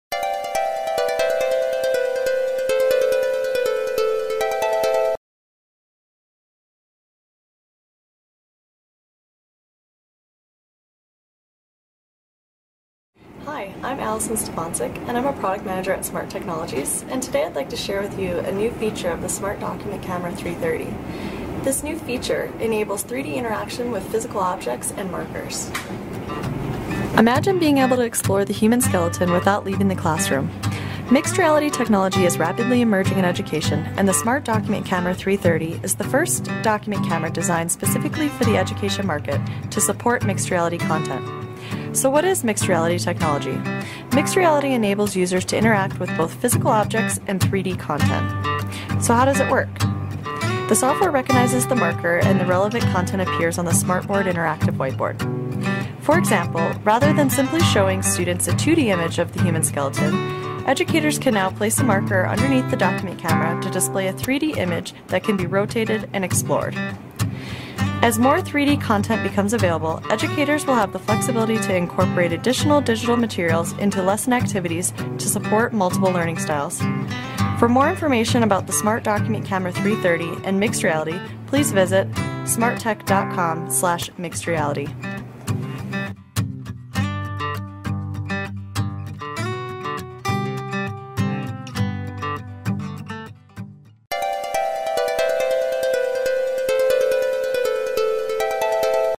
Document Camera Experts - SMART Document Camera 330 SDC-330 Mixed Reality
SMART Technologies announces Mixed Reality document camera for education.  The SMART SDC-330 is an exciting and powerful document camera that integrates directly into SMART Notebook software.  This video was produced by SMART Technologies.